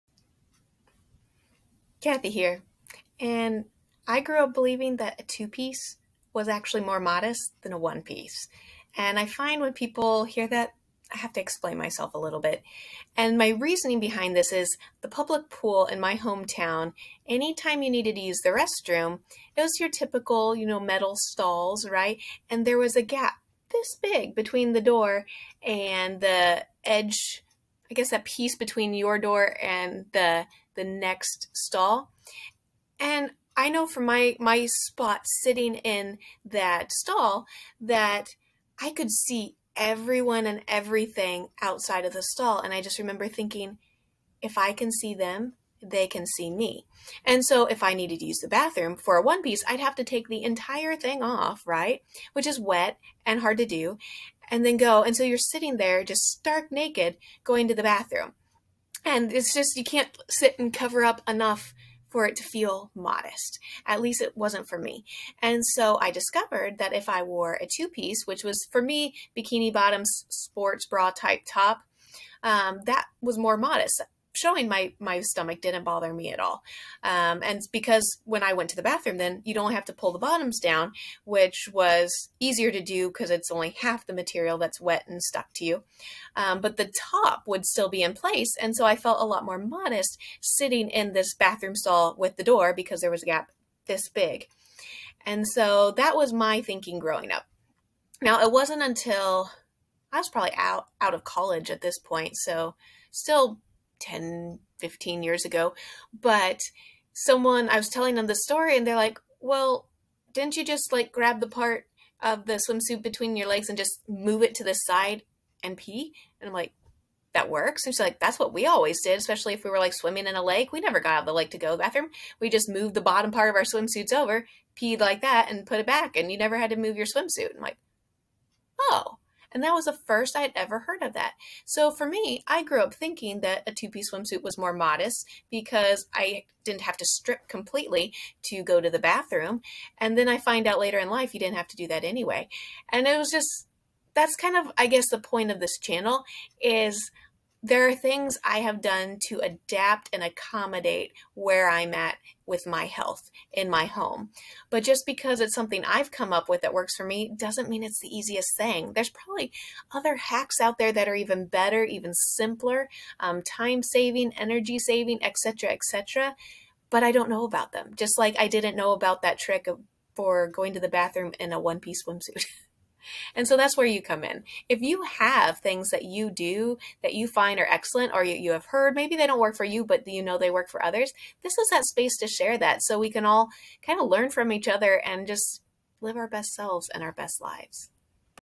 A 2 piece Swimsuit is More Modest
What does the modesty of a 2-piece swimsuit have with this channel? Take a listen, and let me explain ;D